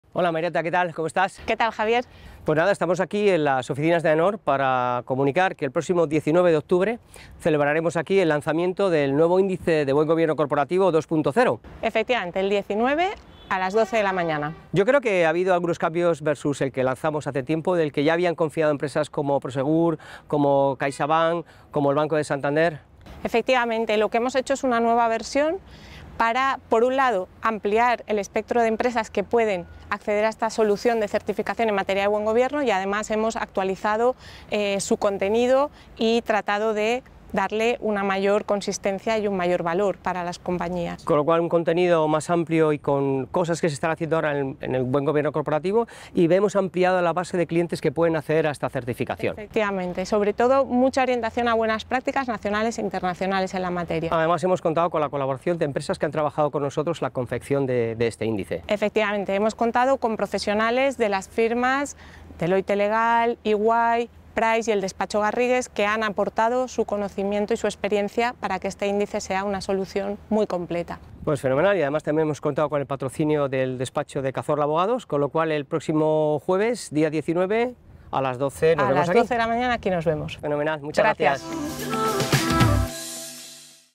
Presentación del nuevo Índice de Buen Gobierno Corporativo 2.0
Nuestro director de Marketing y Desarrollo de Negocio de AENOR, Javier Mejía, y nuestra Mánager de ESG, Mayrata Conesa Alagarda, nos invitan a la Presentación del nuevo Índice de Buen Gobierno Corporativo 2.0.

Una solución de certificación que hemos perfeccionado gracias a la experiencia adquirida y a la colaboración y asesoramiento de entidades de prestigio nacional e internacional como Deloitte Legal, Ernst&Young Abogados, PwC Tax & Legal y el despacho Garrigues.